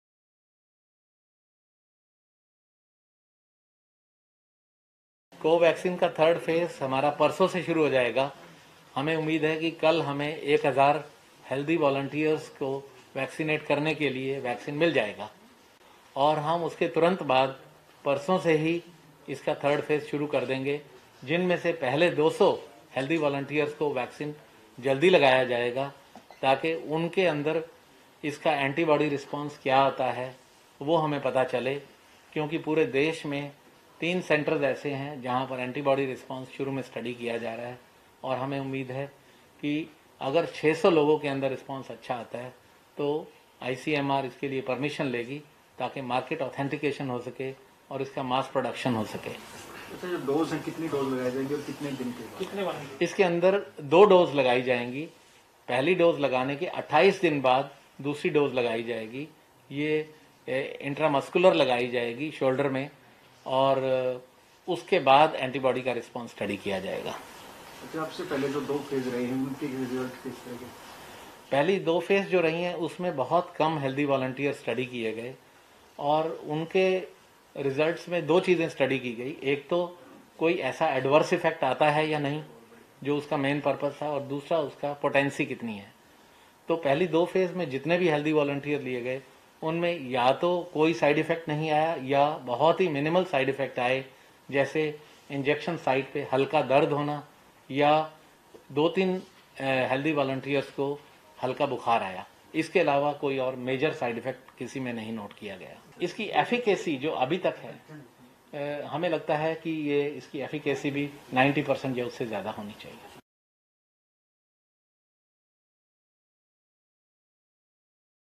Covaxin's efficacy expected to be more than 90%: Dr OP Kalra
Vice-Chancellor of Pt BD Sharma University of Health Sciences, (Rohtak), OP Kalra during a press conference on November 18 said that the third phase of Covaxin will start from November 20.